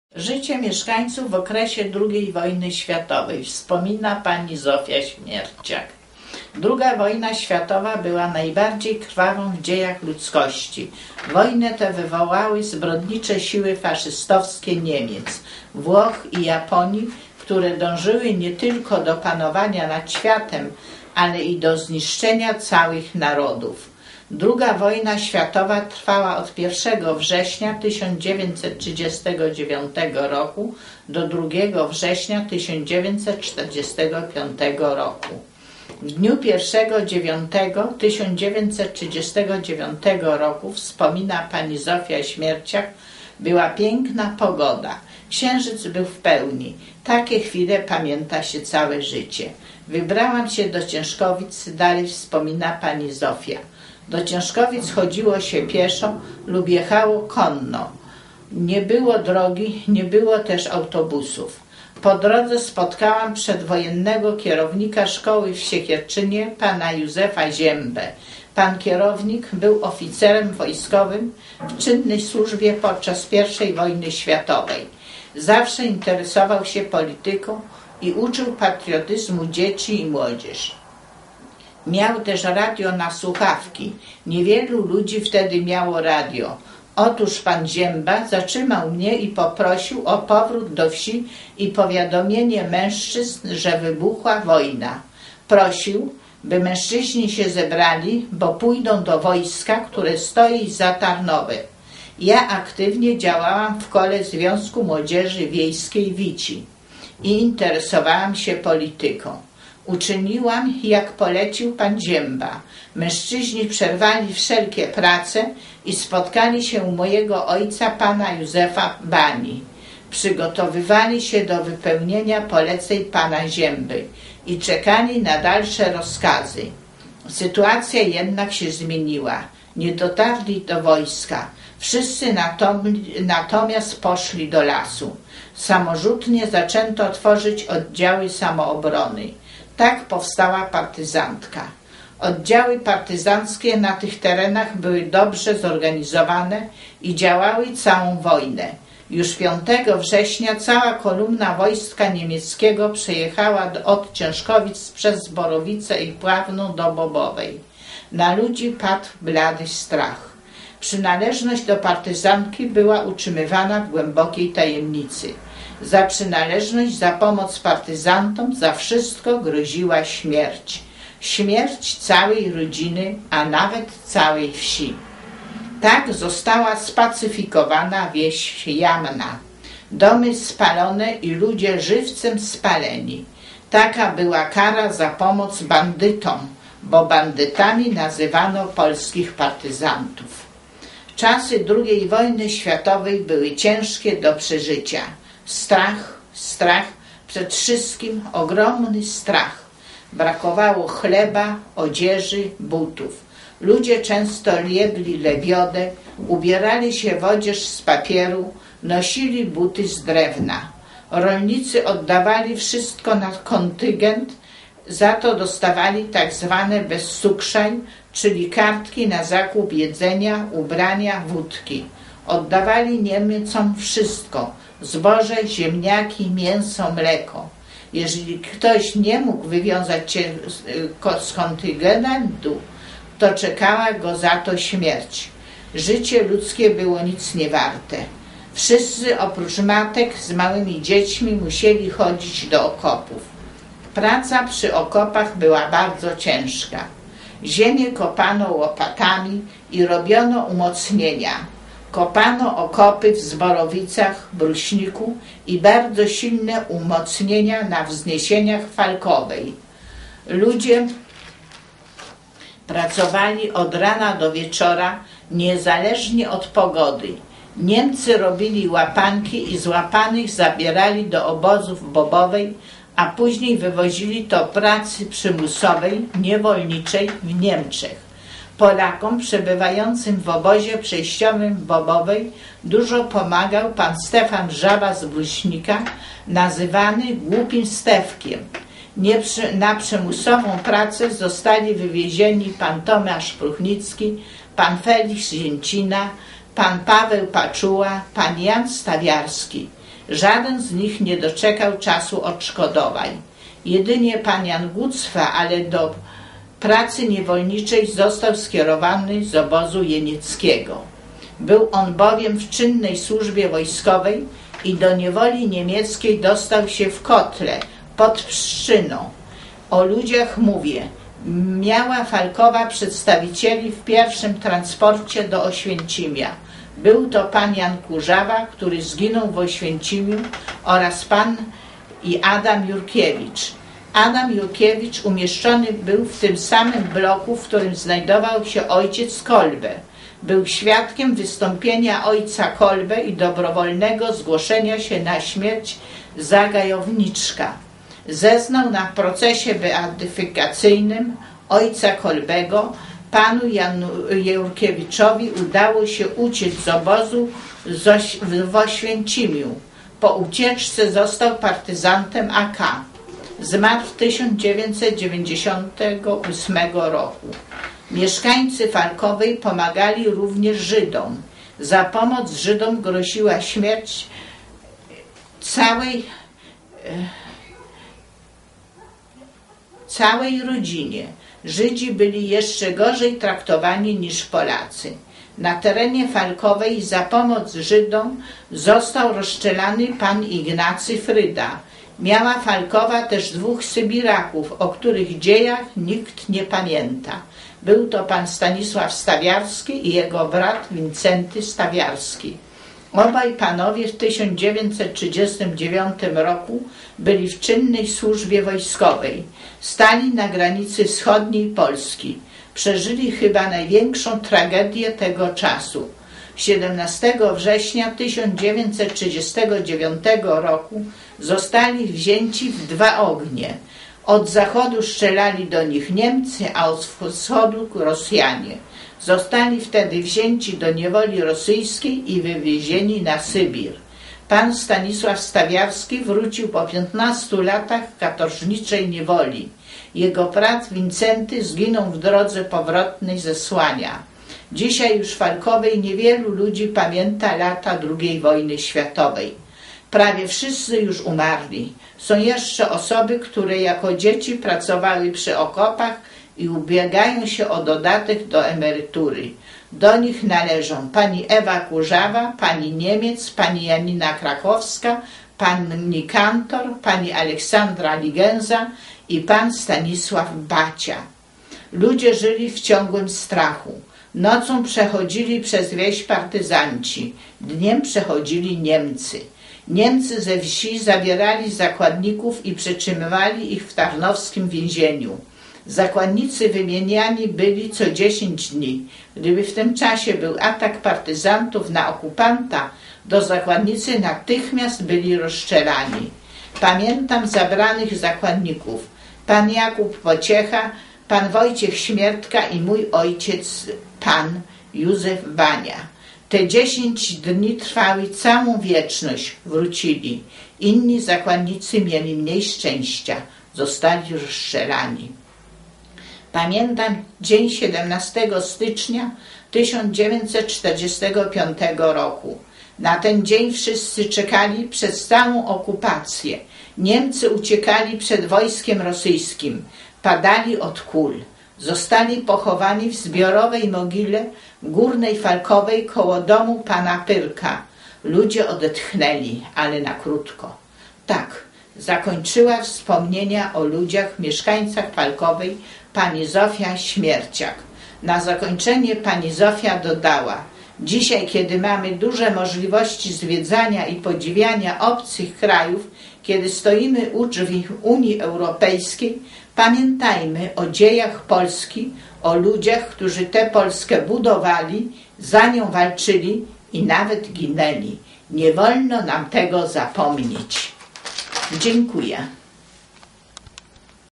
Legendy cz.15 : Falkowa w legendach i historii
Wspomnienia: życie mieszkańców w okresie drugiej wojny światowej